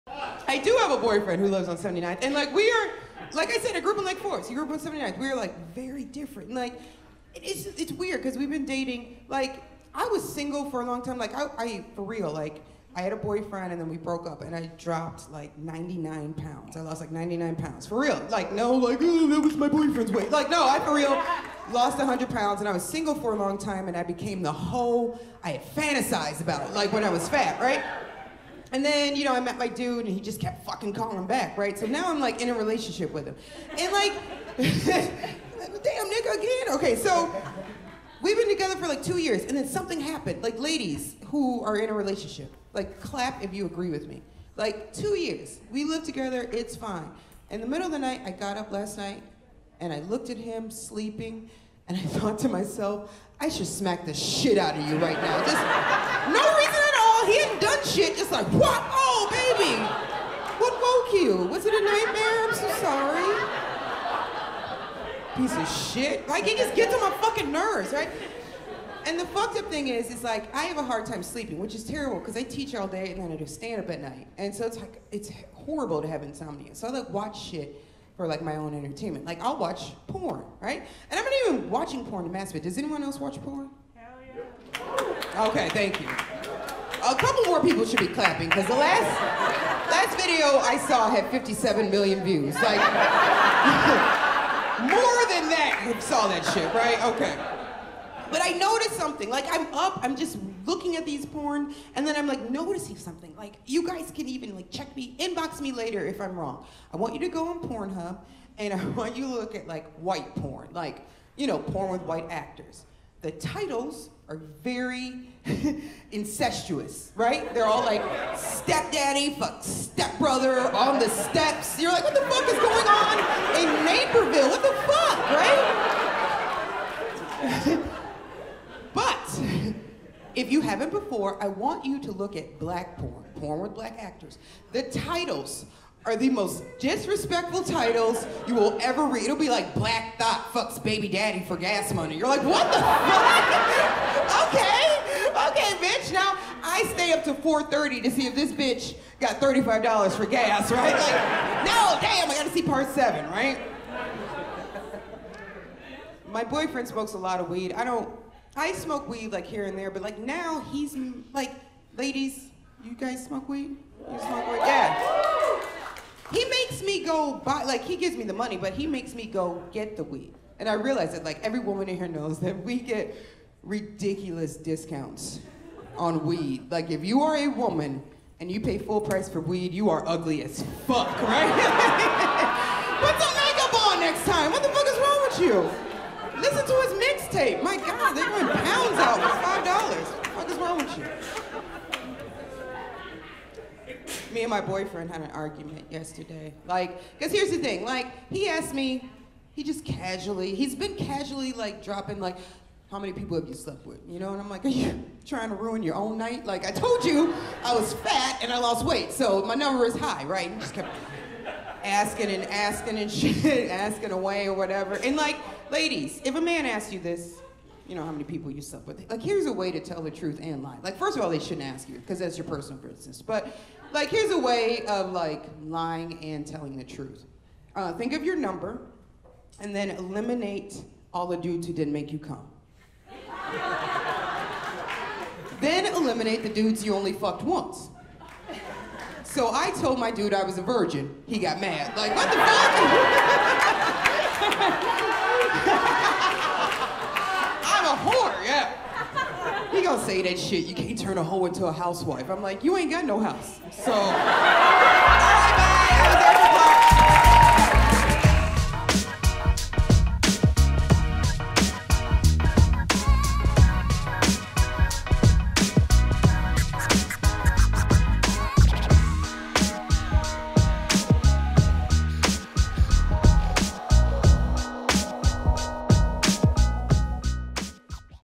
Erica Nicole Clark | Southside Boyfriend | Laugh Factory Chicago Stand Up Comedy
Erica Nicole Clark (@ericanicoleclark) notices how different she is from her Southside Boyfriend. See a LIVE SHOW at the Hollywood Laugh Factory!